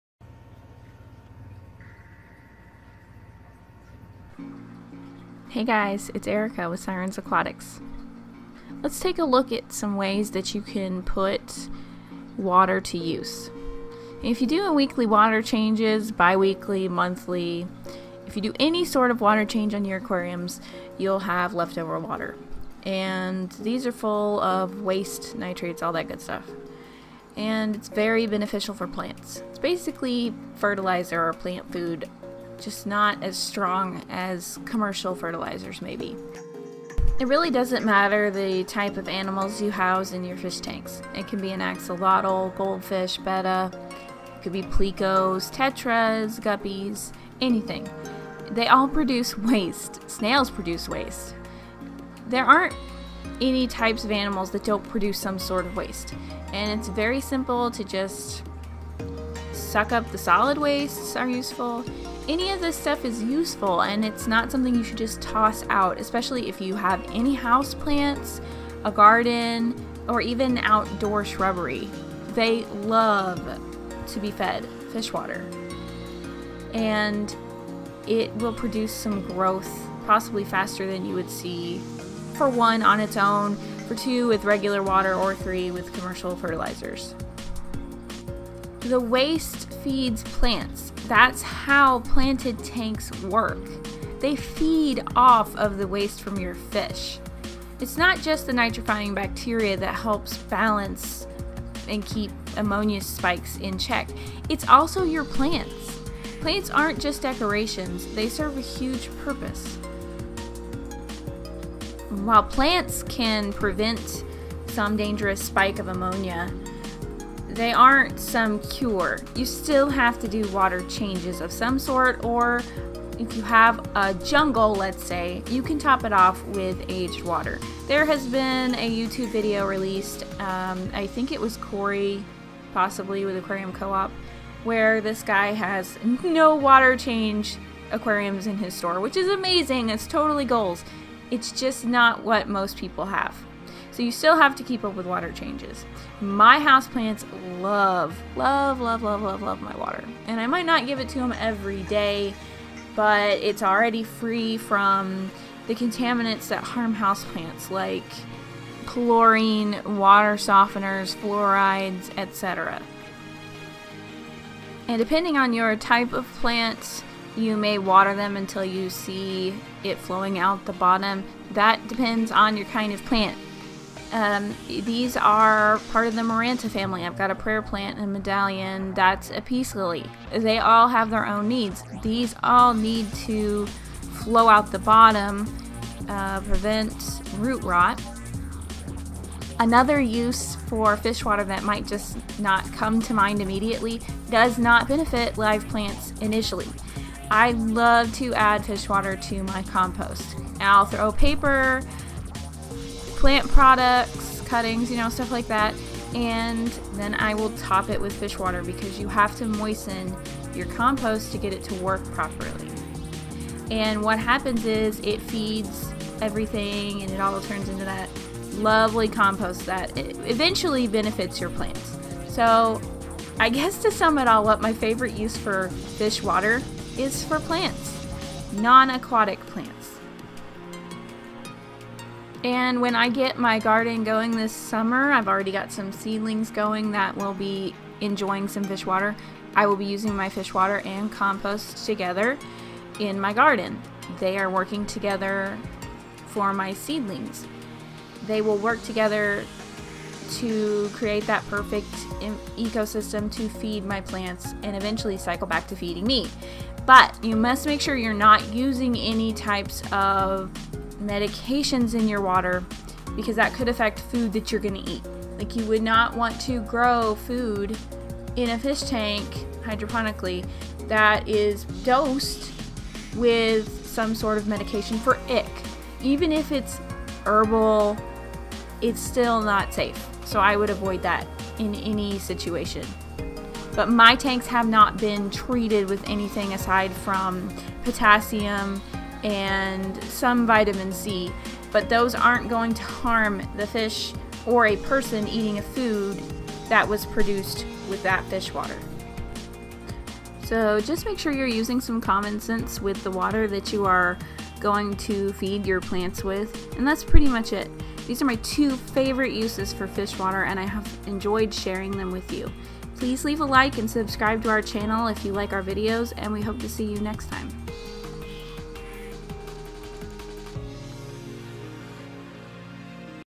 Liquid Gold: Uses for Fish Water after Aquarium Water Changes
Water changes are something most aquarists experience, and complete dilligently on a regular basis. Many simply toss it out, but did you know there are other uses for it? Watch our mini rant on fish tank water uses, before you pour it out!